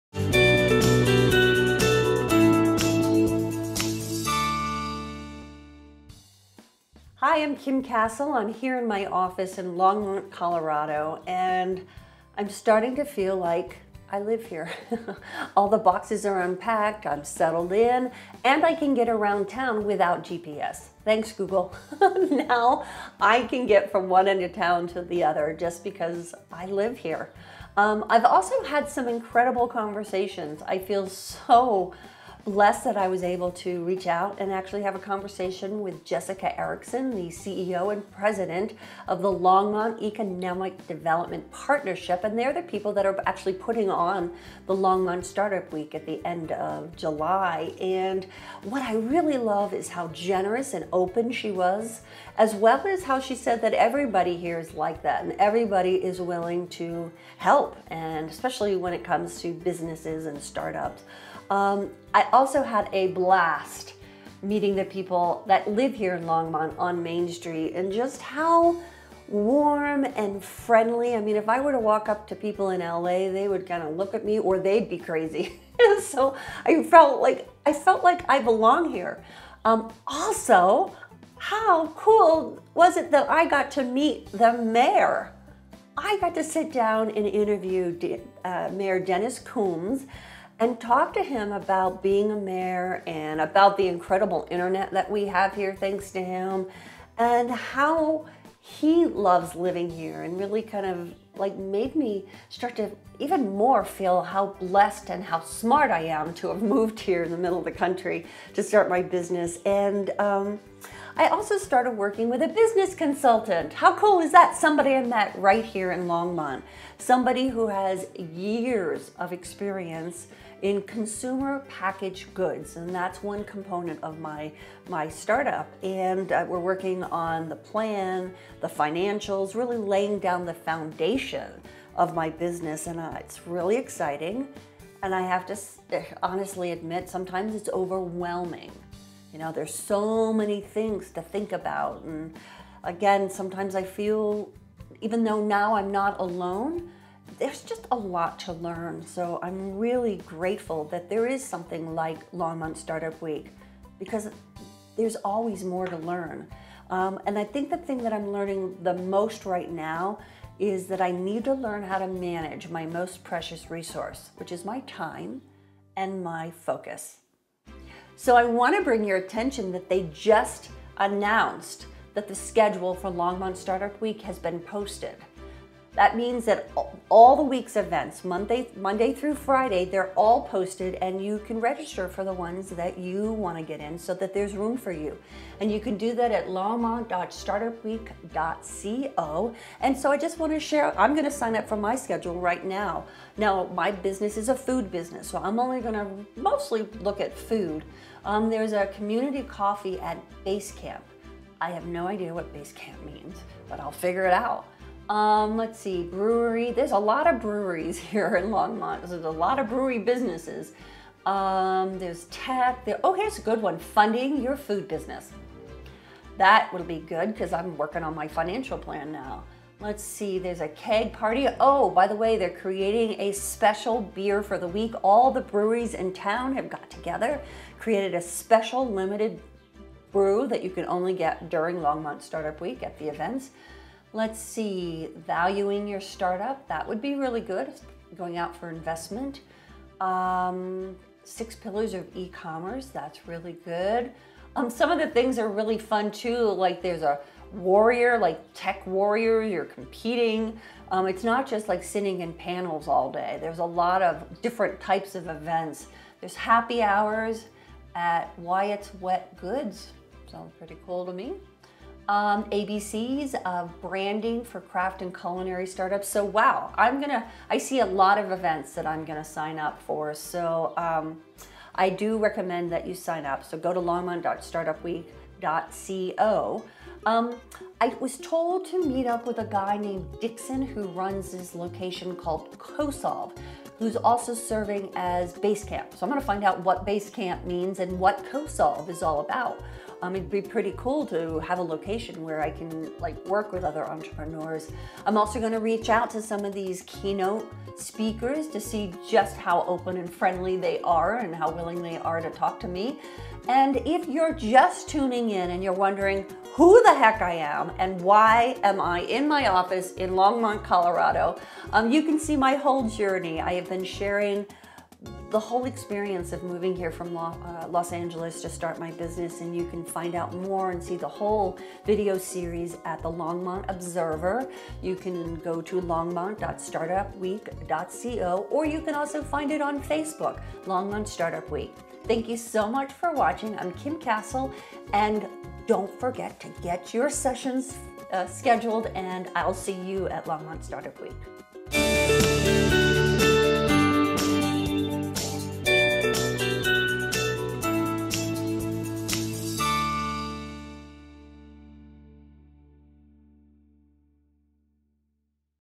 Longmont StartupWeek schedule is live!
LSW17 will bring together entrepreneurs, wanna-preneurs, intrapreneurs, creatives, techies, inventors, business owners, and investors of all ages to connect and inspire.  It’s this support network that can make the difference between closing up shop and pushing through to success.  LSW showcases why Longmont and the northern Colorado region is the best place to start a business and builds momentum in advancing Longmont’s startup community.  Over the course of five days, multiple startup community events, panels, fireside chats, and keynote speaker presentations will be hosted by the community for the community at unique venues throughout Longmont, showcase Longmont and Northern Colorado’s entrepreneurs in advanced technology, I.T., bioscience, creative arts, culinary, and more.  Events will include topics such as design, development, marketing, finance, legal, and entrepreneur basics.  All events are FREE to attend and provide outstanding learning and networking opportunities.